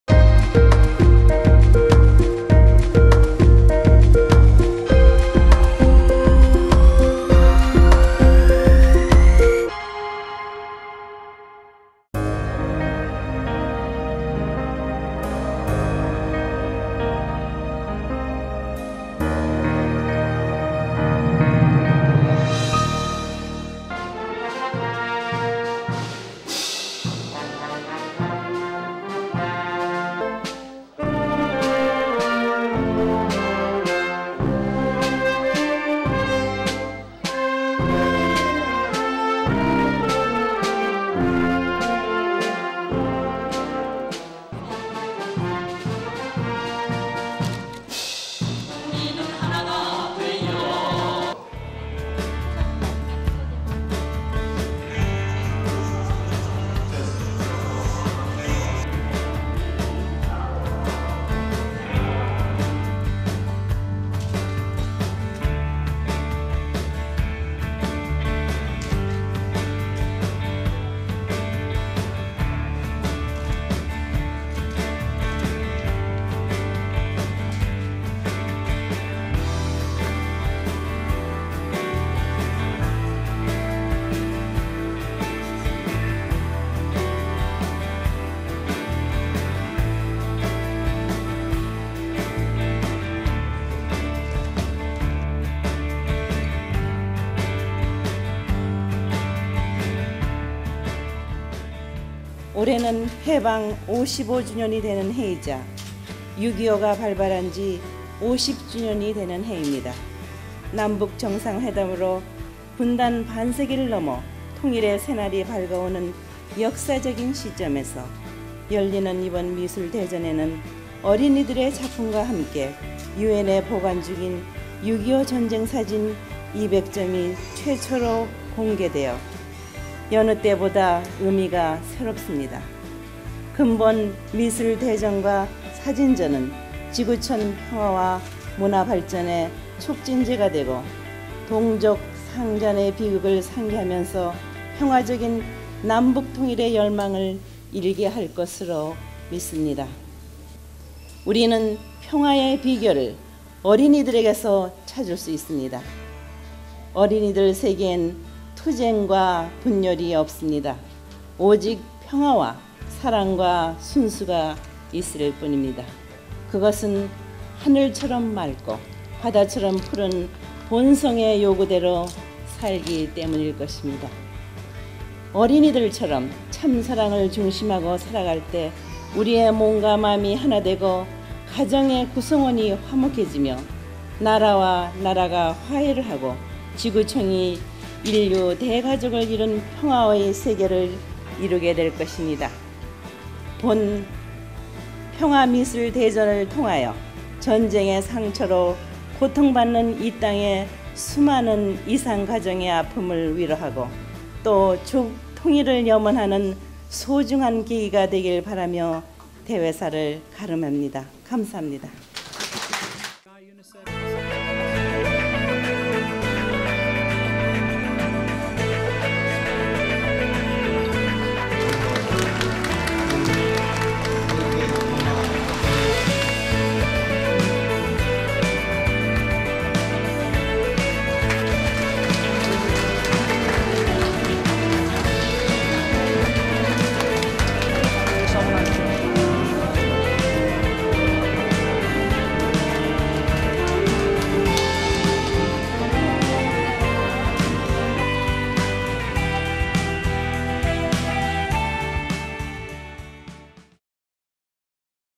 [섭리의 보물창고 디지털 아카이브] 2000년 6월 23일 참어머님 유엔 IAEWP 그랑프리 평화상 수상 및 세계 어린이 평화미술대전 시상식
2000년 6월 23일 참어머님 유엔 IAEWP(세계평화교육자국제연합) 그랑프리 평화상 수상 및 세계 어린이 평화미술대전 시상식

더 많은 영상을 만나고 싶다면 아래 홈페이지 Link를 클릭해주세요.